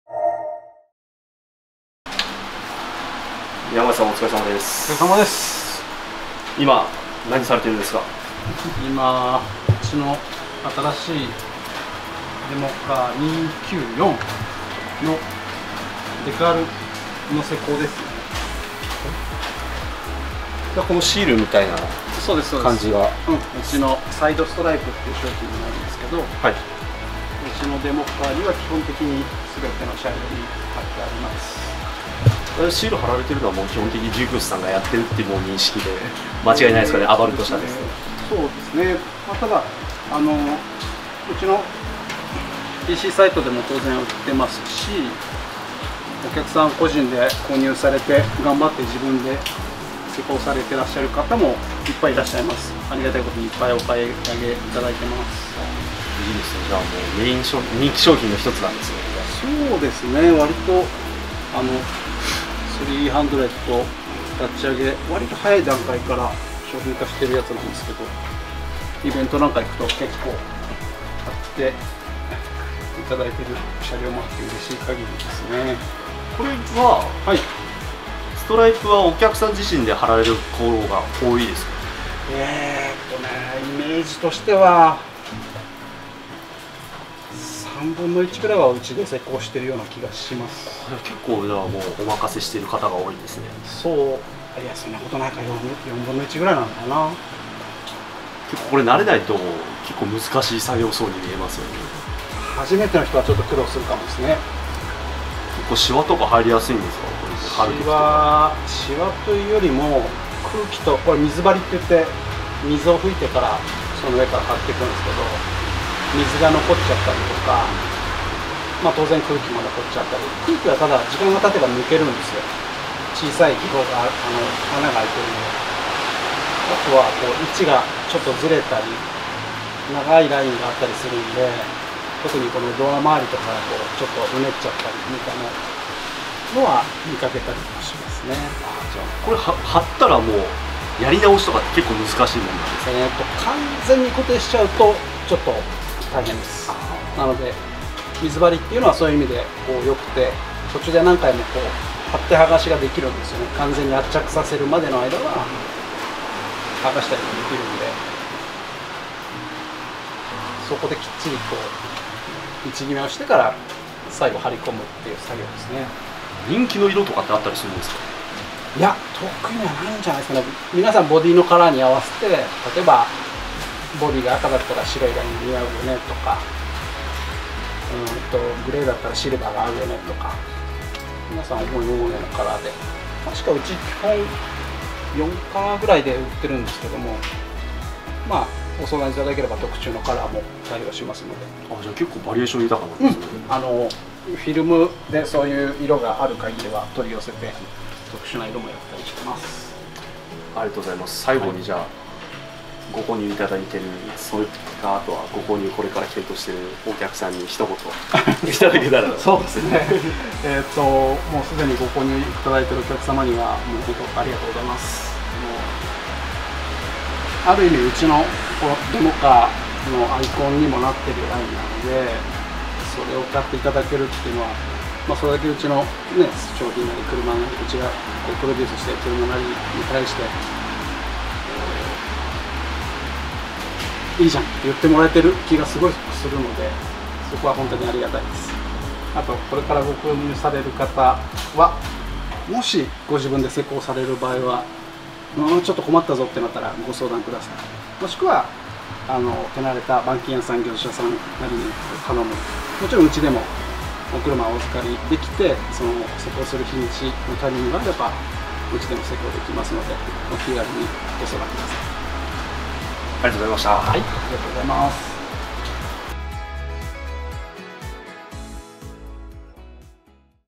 【作業紹介】地味に難しいサイドストライプの貼り方
THREEHUNDREDのブランドアイコンでもあるサイドストライプ。
デモカーをはじめ、多くの方にご愛顧頂いている商品であります。

ご自身で作業をされた事がある方はお分かり頂けると思いますが、ストライプを真っ直ぐ貼るのはかなり難しい作業だったりします。

そのサイドストライプをデザインした本人であるヤマジが、サイドストライプを上手に貼るアドバイスをさせて頂く動画です。
是非参考にして頂けたらと思います。